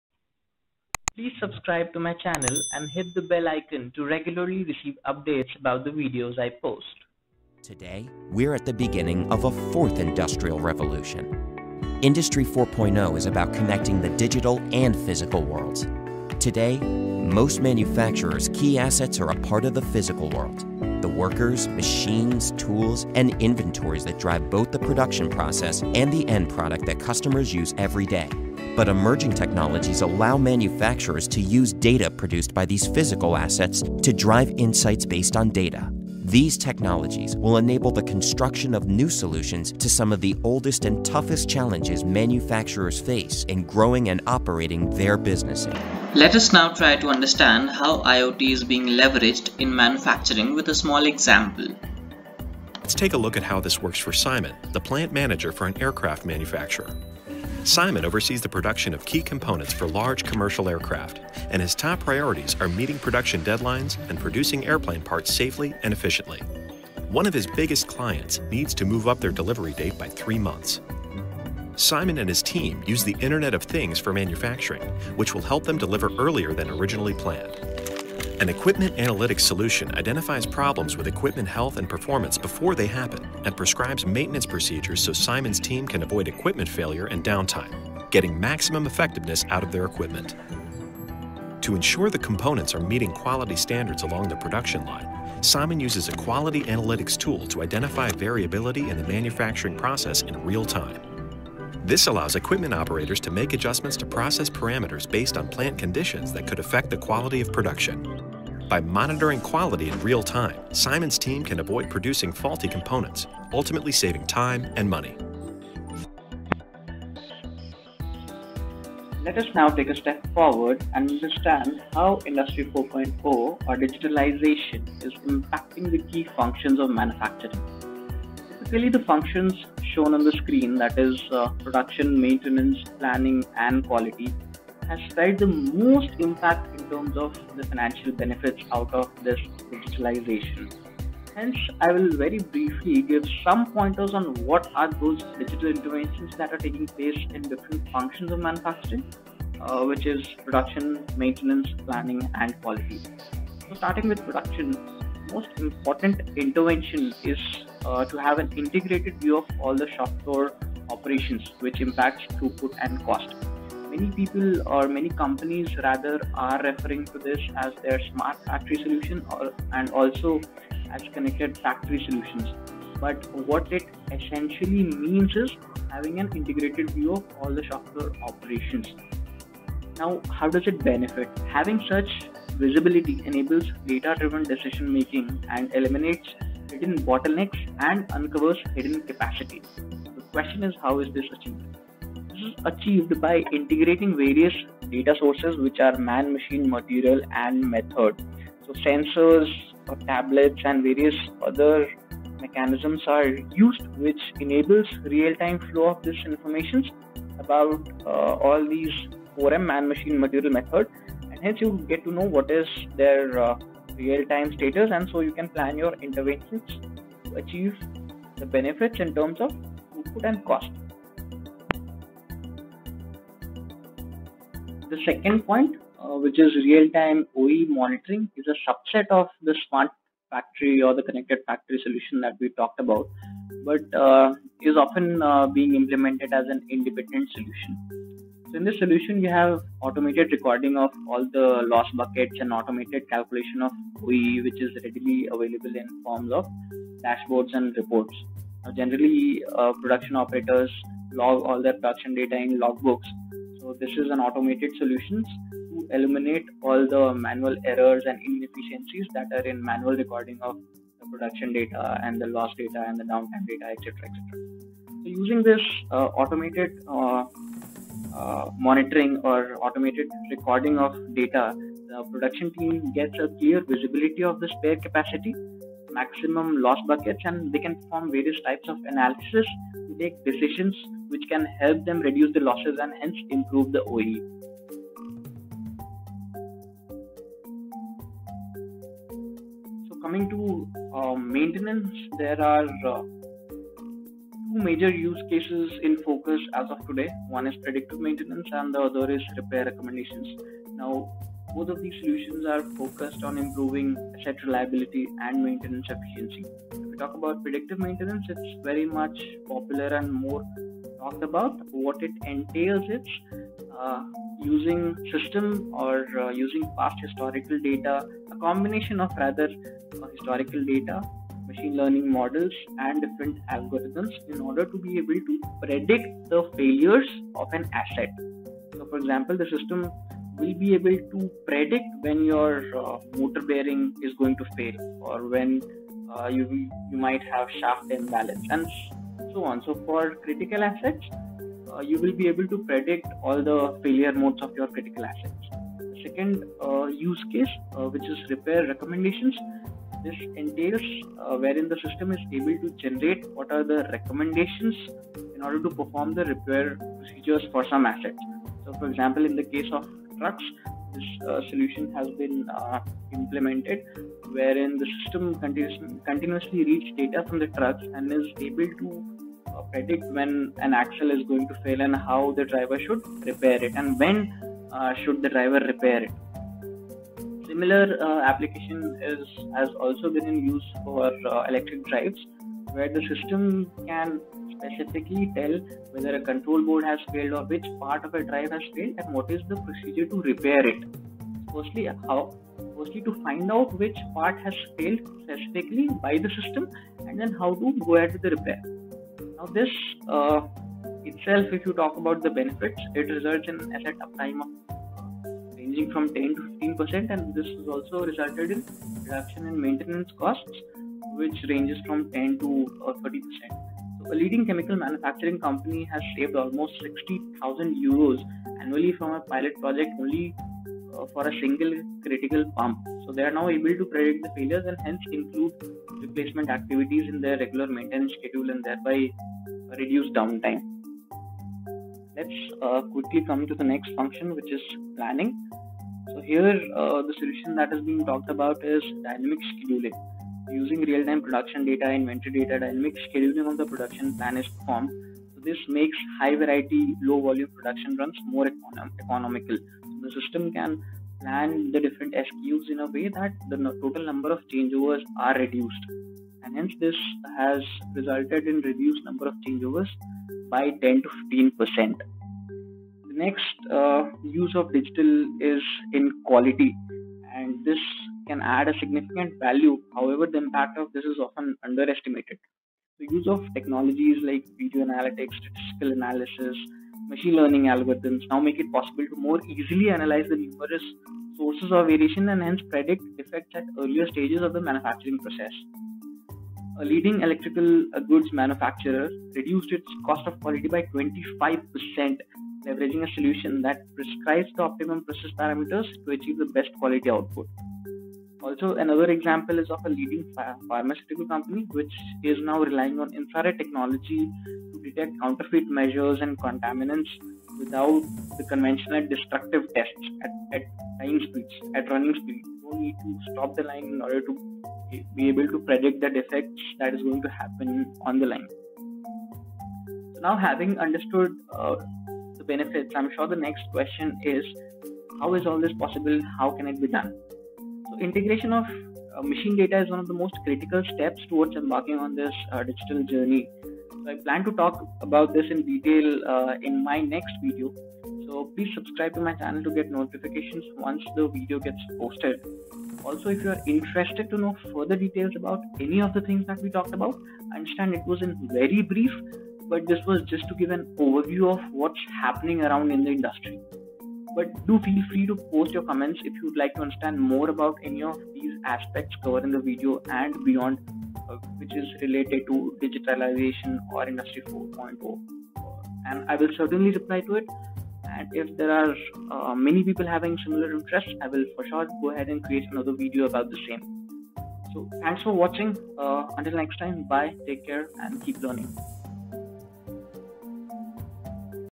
What is IOT or Digitization in Manufacturing ?
Basics about industry 4.0, Internet of things and how digitization/Industry 4.0 is impacting the manufacturing industry.
- Impact of digitalization on production, planning, maintenance and quality.